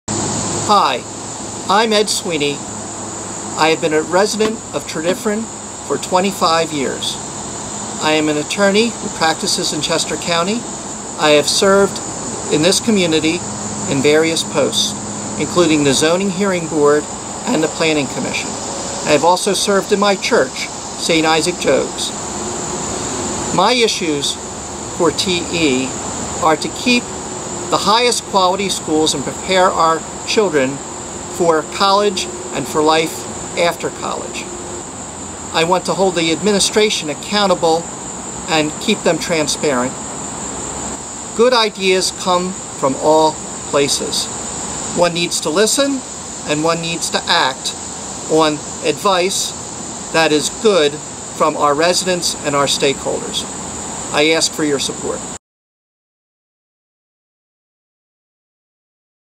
Ed Sweeney for School Board
Ed's bio: What is my background?  I am married to Franny Ryan and we have a 12 year old daughter, Katie, in VFMS.  Franny put her law career on hold to raise Katie.  I own a small law firm, Wusinich & Sweeney, LLC, in Exton, with deep roots in Chester County.  I am a Plaintiff’s attorney who goes up against big insurance companies and corporations.  It makes me particularly sensitive to how big entities treat individuals.  For 25 years, I have served in various volunteer and official roles in the Chester County and Tredyffrin communities and at St. Isaac Jogues.  I value a good education.  I am a graduate of the highly ranked University of Chicago Law School where I met my wife, Franny.  I attended La Salle University on a Christian Brother’s Scholarship.  I graduated St. Joe’s Preparatory School in Philadelphia.  My parents sacrificed for me to attend the Prep and a private elementary school and hoped I would receive a scholarship.

In TESD, we need to preserve and improve what is great (including  a history of moderate taxation, a budget that has not deficit spend, and a history of being welcoming to all) and to correct what can be improved (what I would call charitably “institutional myopia,” most particularly in financial matters).  I believe our School Administration has two “weaknesses” which I have pushed to improve.  First, fiscal matters need to be absolutely transparent and accountable to the Board and through the Board to the public.  Our Board does not have a clear fiscal picture and we have the challenge that our student body has increased 20% in five years.  Second, the Administration can be top down and is perceived, rightly in my view, as often resistant to reasonable suggestions from the public-at-large.

I ask humbly for your support and would advocate my candidacy as follows.  First, I will have had four years on-the-job Director experience as a productive board member dealing with often very difficult issues.  I am familiar with what has been done and what needs to be done.  Being on a volunteer Board is a unique skill set and I have many years of service on volunteer boards.  I am good at it.  I respect good ideas and listen for them.  Second, I recognize and continue to be the most major contributor on trying to address a weak spot at TESD – an occasional “institutional myopia.” I do so carefully because it is obvious we have one of the best public educational experiences in many areas in the nation.  This is my “role” on the Board and my job is a work-in-progress.  Third, I am an advocate and negotiator from a conservative perspective.  I fight for taxpayers, retired individuals and single income families.  If I can’t make a good tax deal, I vote against a budget.  TESD’s affordability is the key to attraction to our community.  Our fiscal prudence is a challenge.  Fourth, I have upheld the dignity and gravitas of this School Board in this professional community.  Board Directors set the example for our students, teachers, and administrators.  If elected governance can’t work here, it can’t work anywhere.  Finally, I want to make public schools comfortable for all students and families.  However, I draw the line at  proselytization. I have fought to keep partisan ideology out of our schools.

It has been thus a very great honor to try to serve the public well in the historic Valley Forge area as an elected School Director.   I try to serve in a way that brings credit to the district even when I push for improvement.  This district is exceptional.  I love interacting with the highly skilled people we hire, my talented colleagues, and the knowledgeable community members.